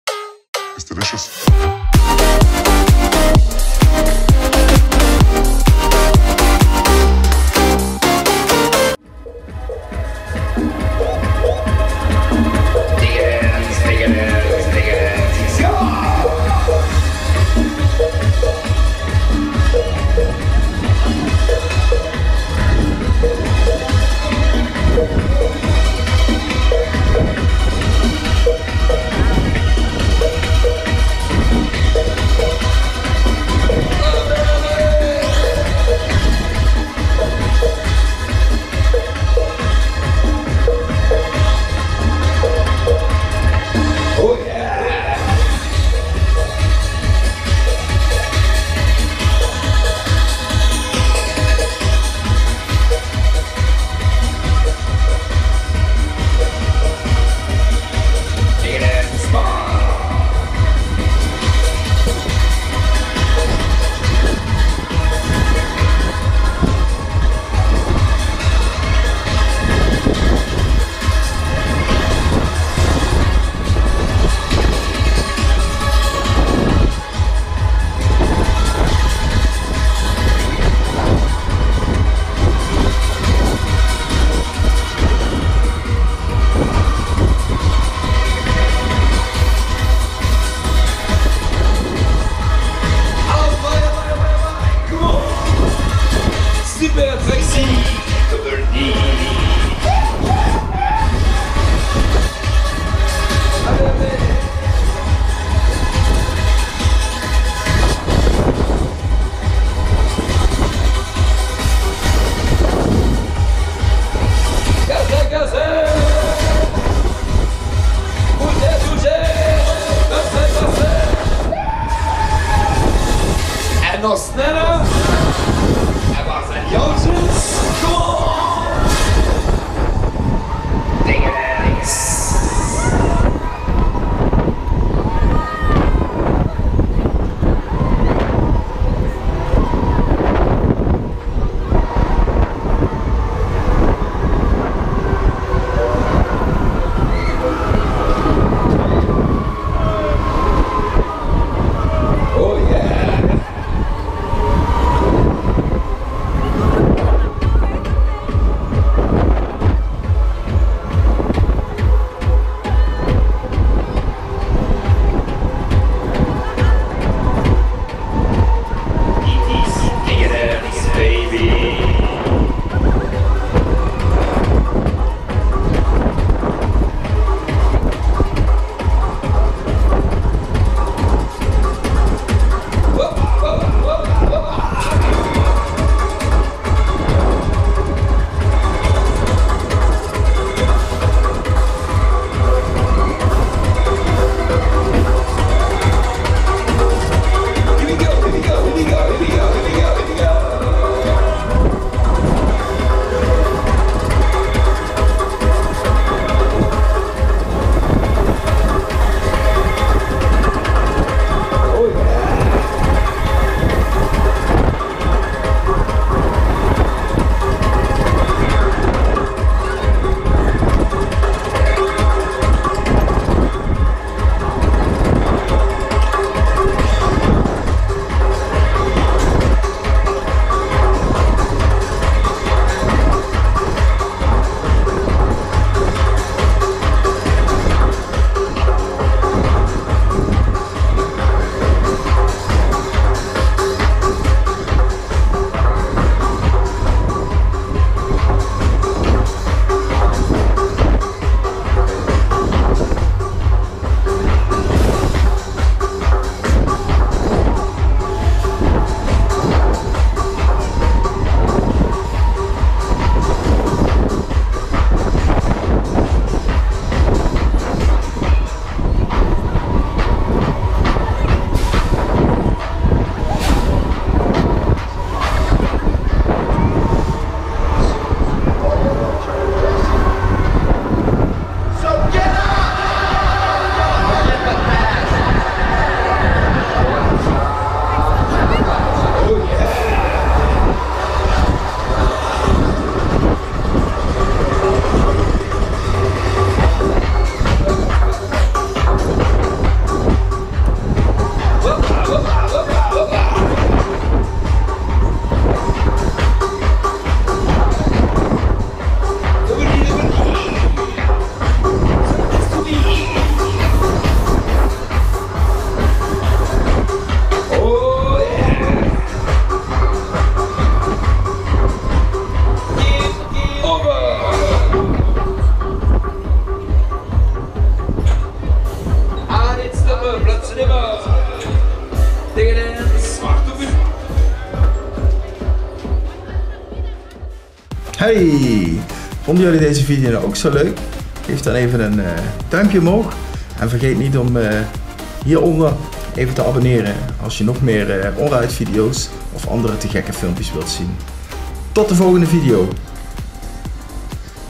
ONRIDE Deca Dance @ night (Verwijk), Kermis Hasselt 2017
Onride video van de Deca Dance hieronder alle details:

★★★Exploitant (operator | Schausteller | opérateur)★★★
Piet Verwijk [Beveren, BE]

★★★Fabrikant (manufacturer | Hersteller | fabricant)★★★
Huss (D)

★★★Type (Typ)★★★
Breakdance no. 1

★★★Bouwjaar (year of construction | baujahr | année de construction)★★★
1985

★★★Afmeting (size | maße | la taille)★★★
(LxBxH):  20 x 20 x 14 m

★★★Aansluitwaarde (connection value | anschluss | valeur de connexion)★★★
3x 125 amp

★★★Capaciteit (capacity | kapazität | capacité)★★★
32 Personen, 16 gondel per 2 personen

★★★Vorige eigenaren (previous owner | vorbesitzer | propriétaire précédent)★★★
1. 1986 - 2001 Mark Bufkens [BE]
2. 2002 - heden Piet Verwijk [Beveren, BE]

★★★Vorige namen (previous names | ehem. namen | noms précédents)★★★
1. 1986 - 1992 Mark Break Dance
2. 1992 - heden Deca Dance

★★★Premiere★★★
1986 Kermis Aalst

Volg jij ons al?